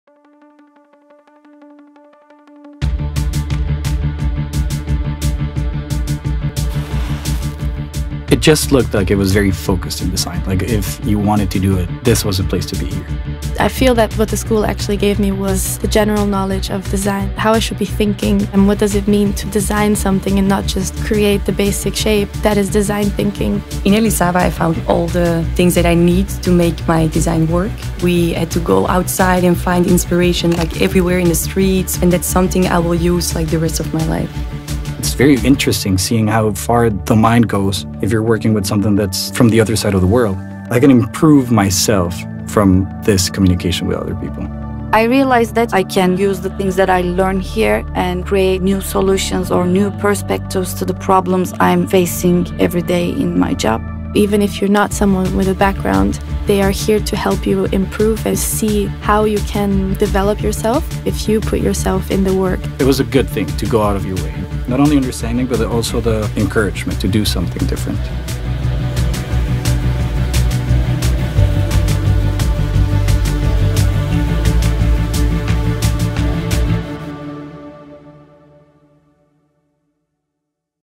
Study Abroad Elisava
How to study abroad? At Elisava we have a 15-week programme in Barcelona, Study Abroad, which offers a comprehensive view of design. Are you looking for design courses abroad? If you want to study abroad in Barcelona, Elisava is the perfect option for you!

If you want to live a study abroad experience in Spain, Elisava Study Abroad offers to our international students courses such as interior spaces, typography, graphic design, furniture and product design, retail design and visual merchandising, motion graphics and visual communication, packaging design or food design.

¿Cómo estudiar en el extranjero? En Elisava contamos con un programa de 15 semanas en Barcelona, Study Abroad, que ofrece una visión integral del diseño. ¿Estás buscando cursos de diseño en el extranjero? Si quieres estudiar en el extranjero en Barcelona, ¡Elisava es la opción perfecta para ti!

Si quieres vivir una experiencia de estudio en España, Elisava Study Abroad ofrece a nuestros estudiantes internacionales cursos como espacios interiores, tipografía, diseño gráfico, diseño de mobiliario y producto, diseño de retail y visual merchandising, motion graphics y comunicación visual, diseño de packaging o food design.

Si estás pensando en estudiar diseño en el extranjero, especialmente estudiar en España, aquí tienes más información